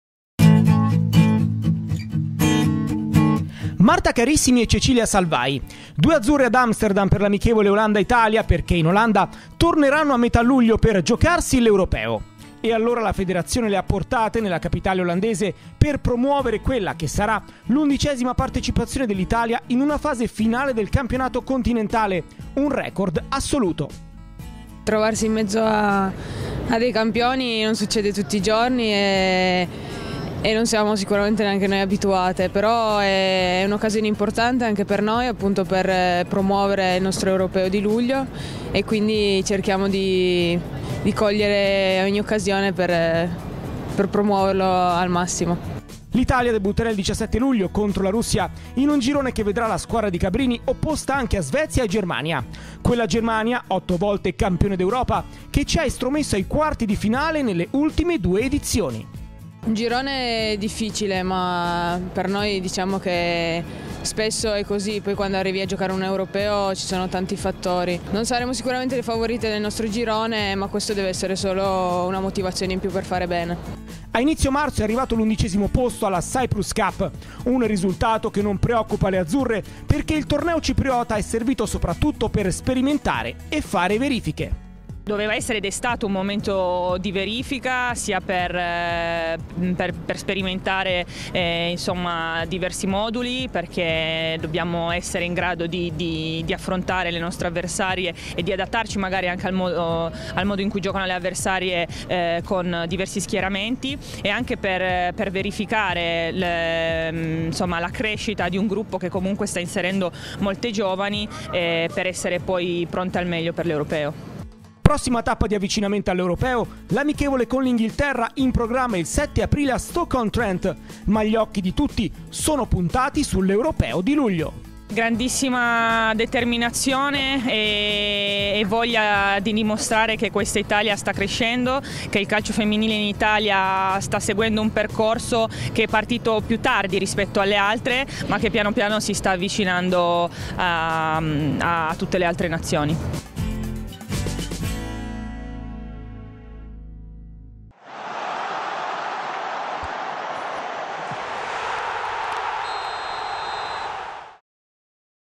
Women's EURO 2017, Carissimi e Salvai in Olanda per promuovere la Nazionale
Le due Azzurre hanno trascorso due giorni ad Amsterdam in occasione dell'amichevole della Nazionale maschile con l'Olanda per promuovere la partecipazione della Nazionale femminile agli Europei che si terranno dal 16 luglio al 6 agosto proprio in Olanda